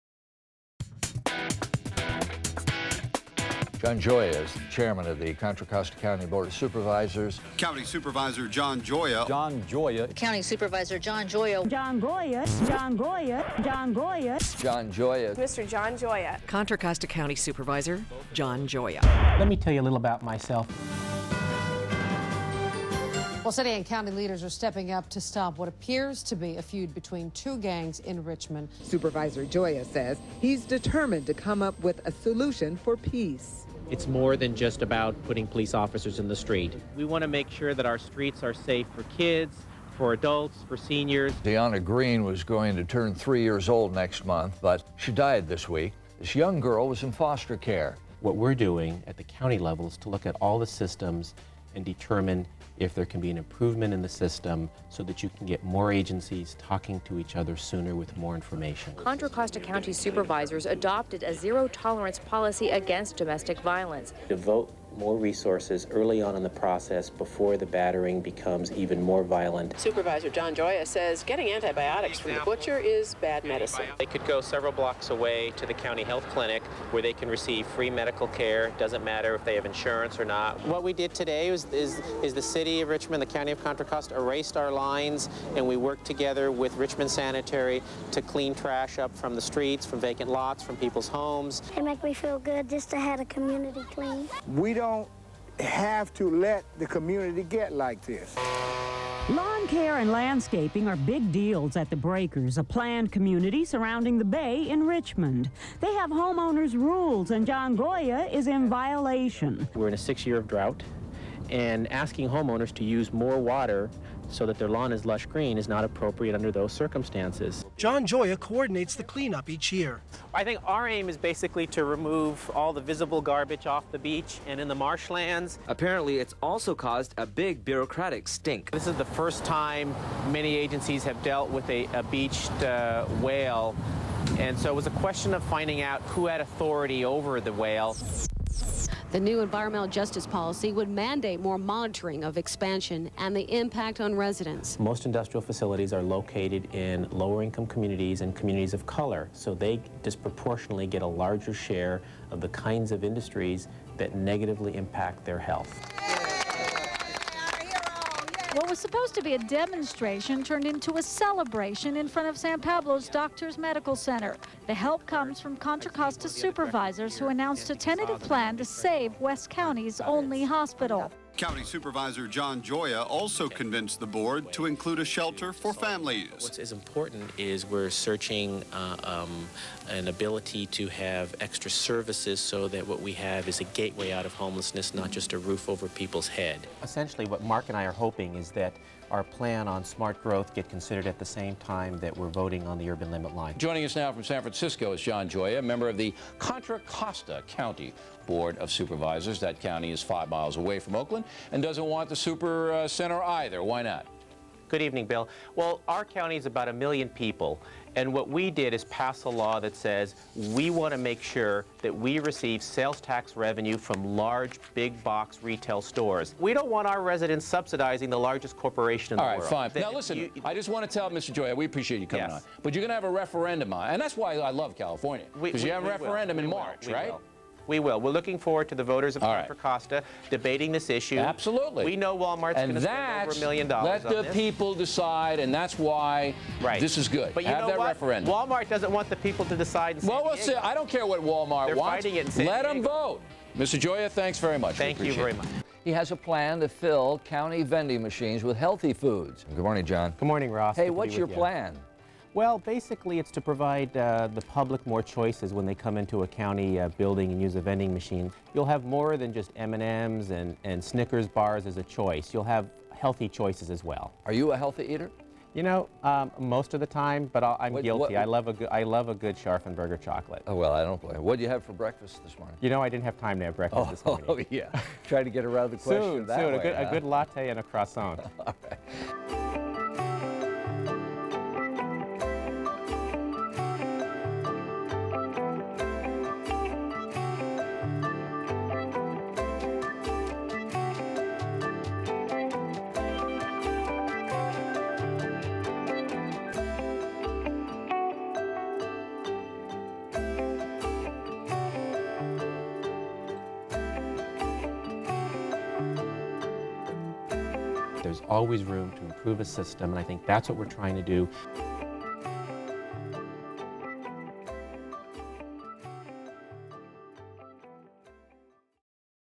Supervisor John Gioia in the News
News Clips of Contra Costa Supervisor John Gioia from the past 20 Years